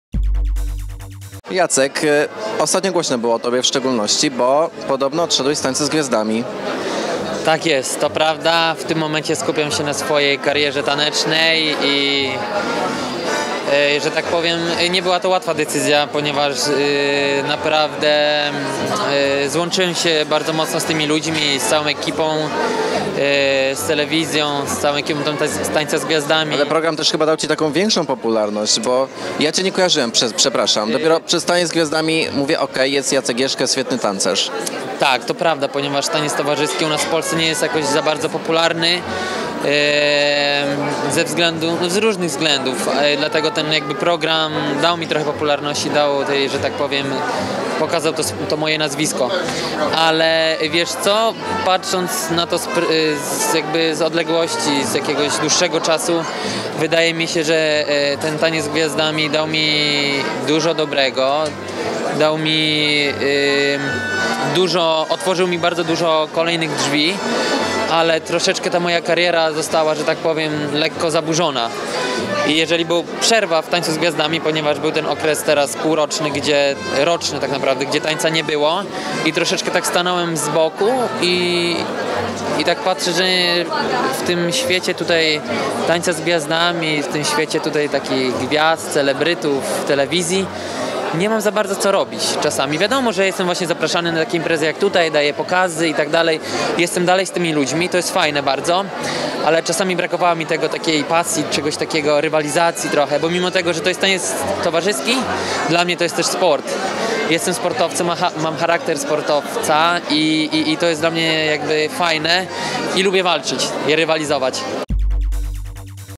Dlaczego Jacek Jeschke odszedł z "Tańca z Gwiazdami"? | przeAmbitni.pl
25-lecie Radia Kolor - 27.01.18 r.